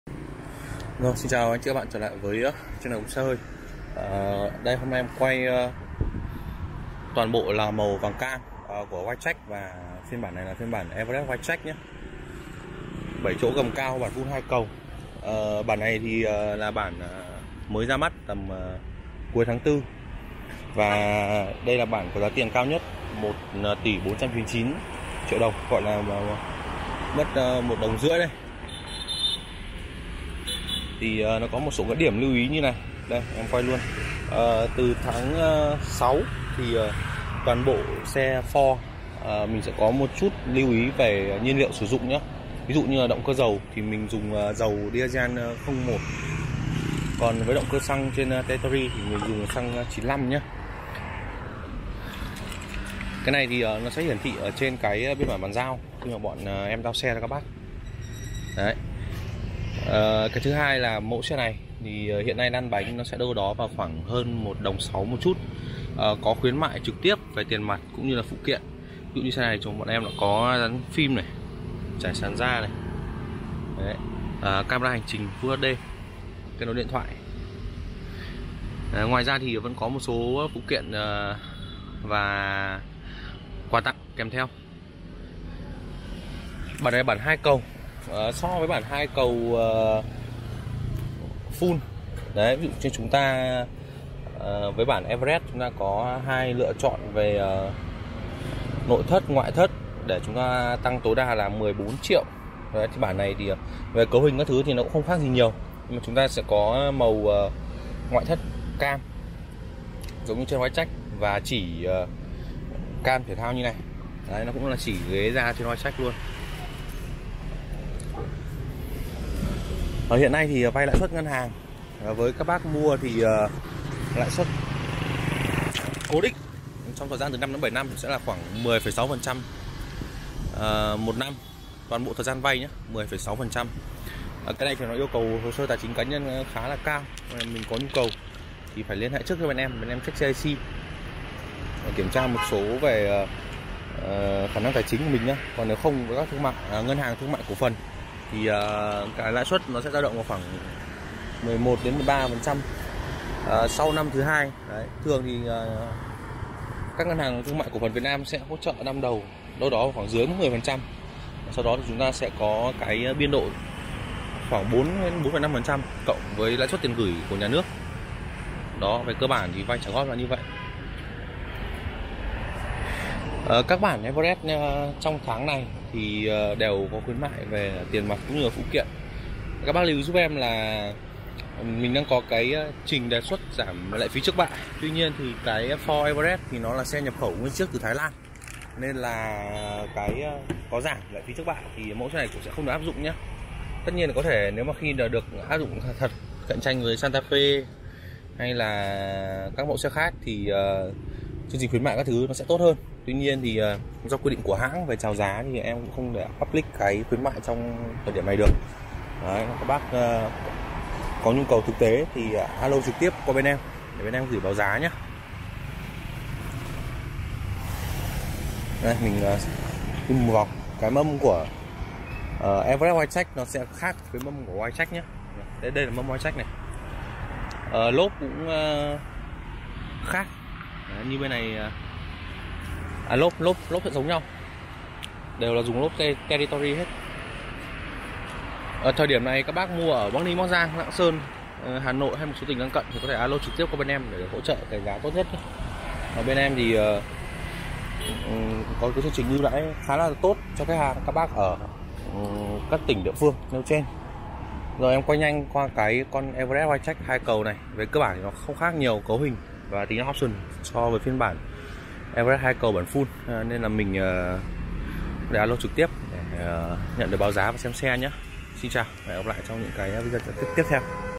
Ford Everest Wildtrak 2023 Lên Phụ Kiện Cơ Bản | Giá Lăn Bánh Tháng 6 Tại Bắc Ninh, Bắc Giang
+ Hotline Tư Vấn: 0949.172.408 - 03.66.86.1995